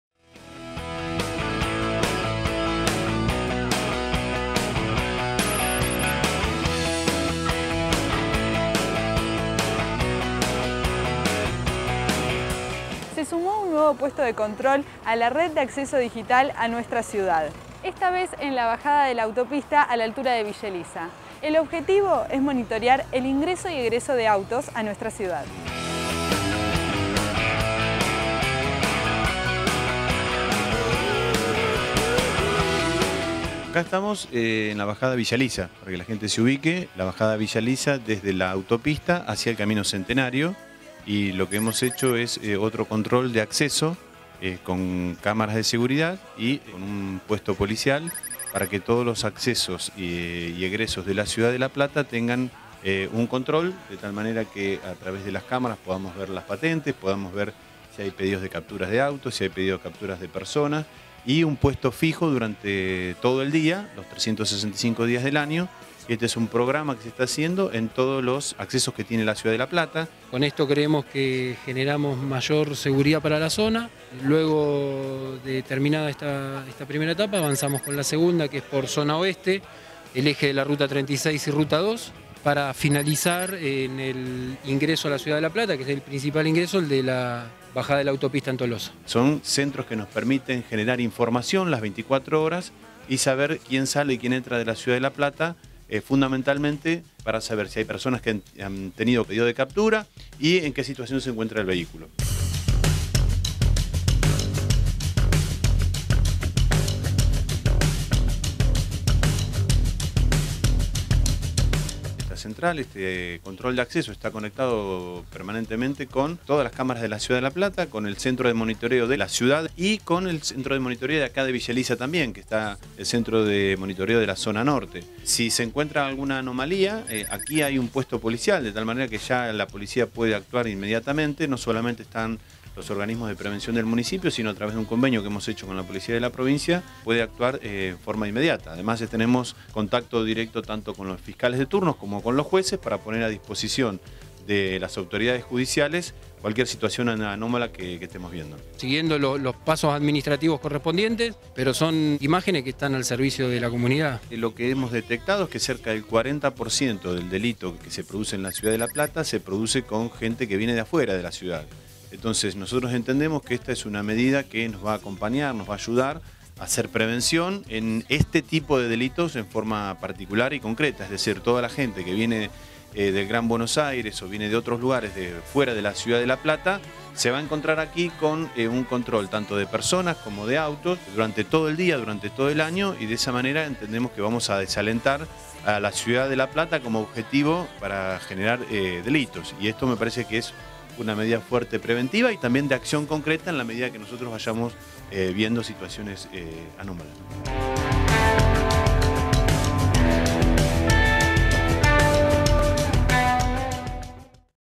Nuevo puesto de control de la Red de Acceso Digital (RAD) en Villa Elisa
Tecnología al servicio de la seguridad: control vehicular en ingresos y egresos a la Ciudad.

Mirá la nota y enteráte de las novedades sobre la Red de Acceso Digital (RAD) en La Plata.